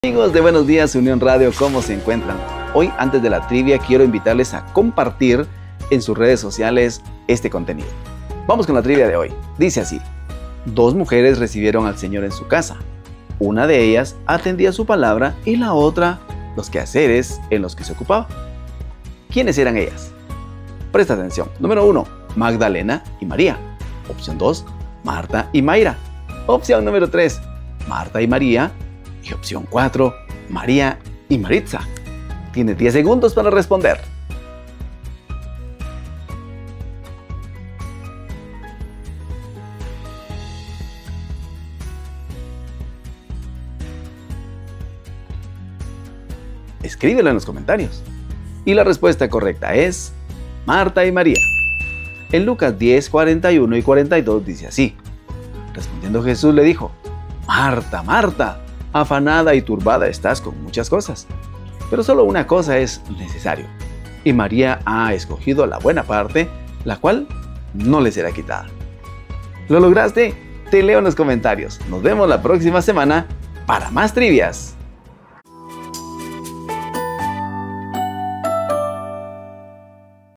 BUENOS DÍAS UR - TRIVIA CRISTIANA
¡Es hora de la trivia bíblica! 🙌🏼 Hoy vamos a recordar a dos mujeres muy especiales que recibieron a Jesús en su casa. 🤔 ¿Podrás adivinar quiénes eran?

Haz tu mejor intento y déjame tu respuesta en los comentarios👇 ¡Tienes 10 segundos para participar! ⏳

Luego me cuentas si acertaste o si esta te hizo pensar un poco más 😅 ¡Nos vemos en los comentarios!